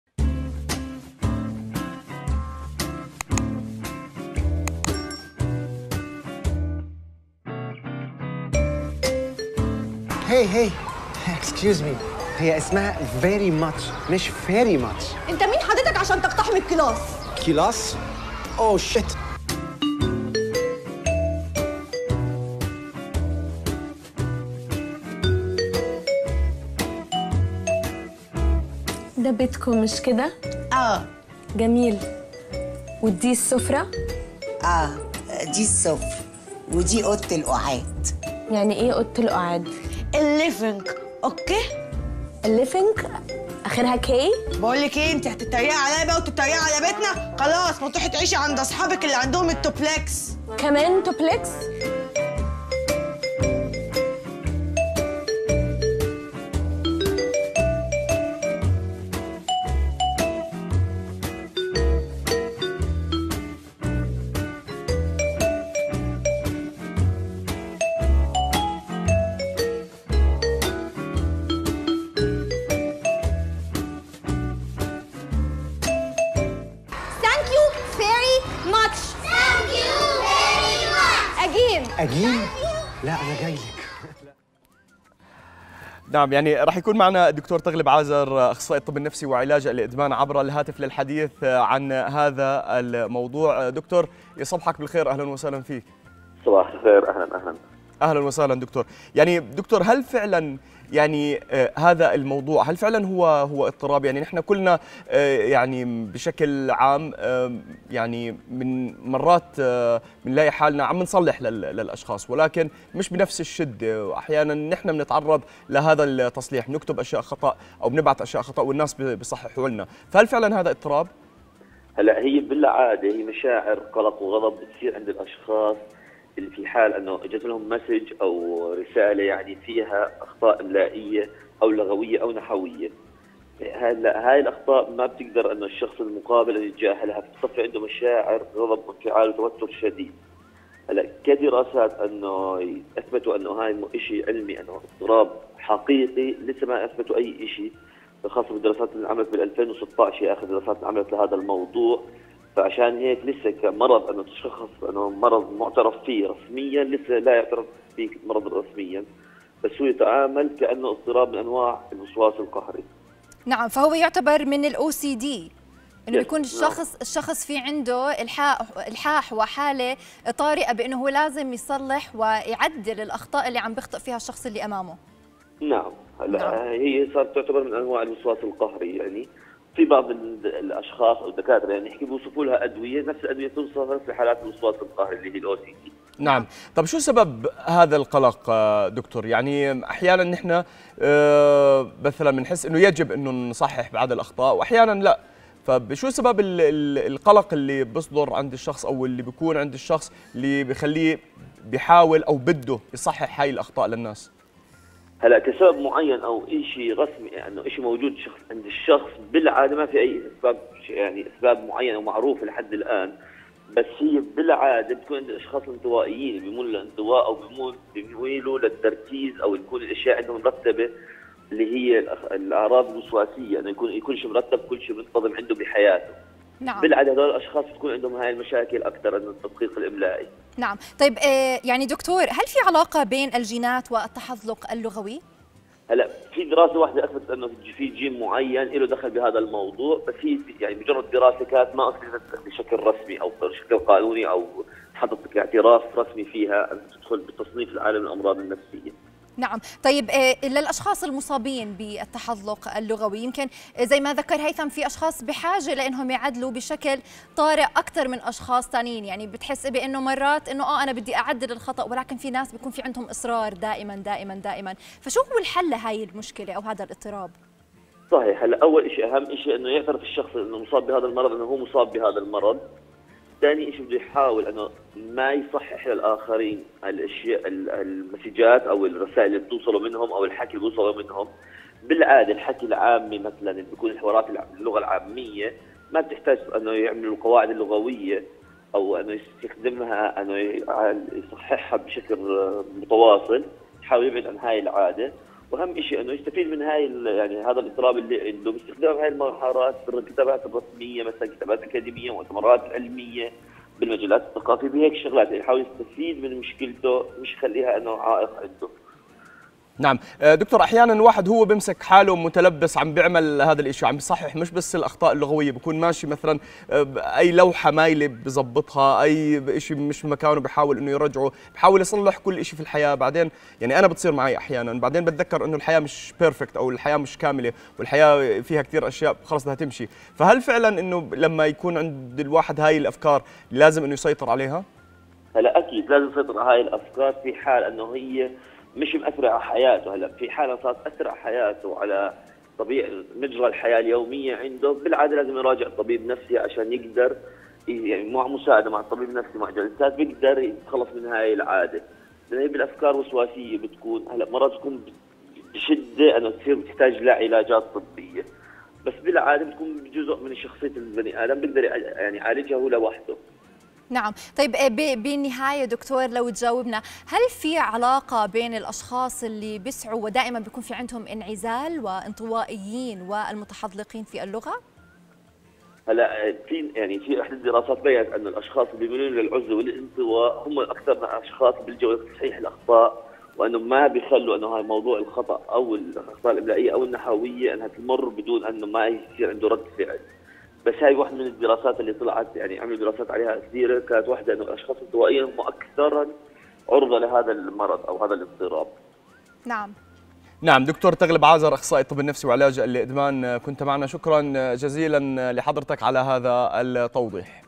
ما هي متلازمة التحذلق اللغوي ؟
د. تغلب عازر- اخصائي الطب النفسي وعلاج الادمان تحدث عن متلازمة التحذلق اللغوي